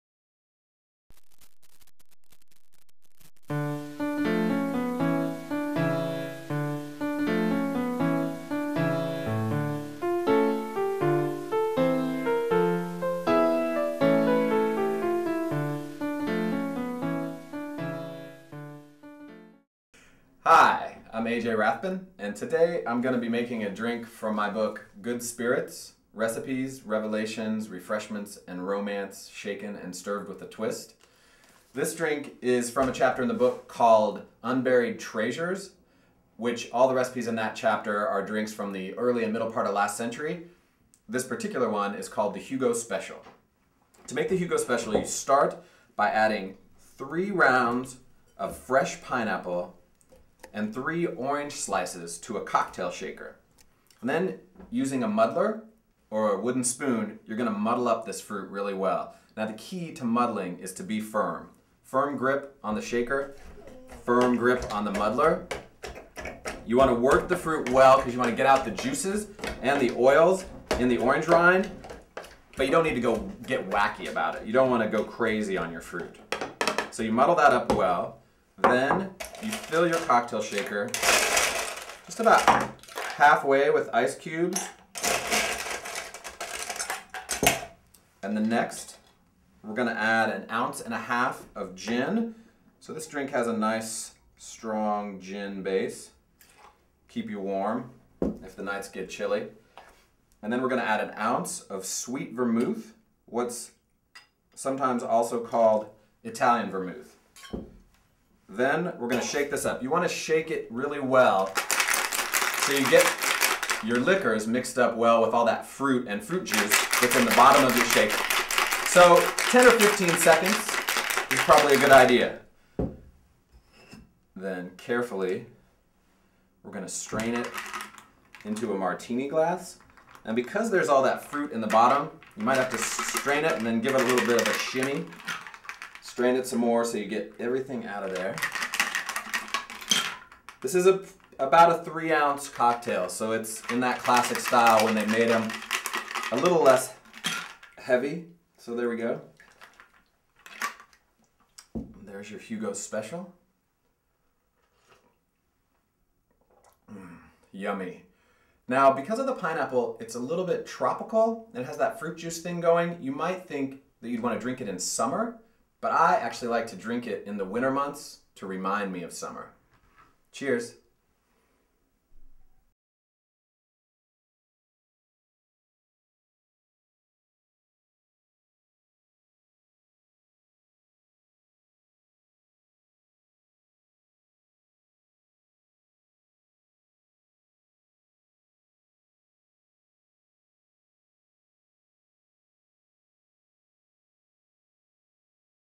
A.J. Rathbun - Good Spirits - Hugo
A.J. Rathbun makes a Hugo which is featured in Good Spirits.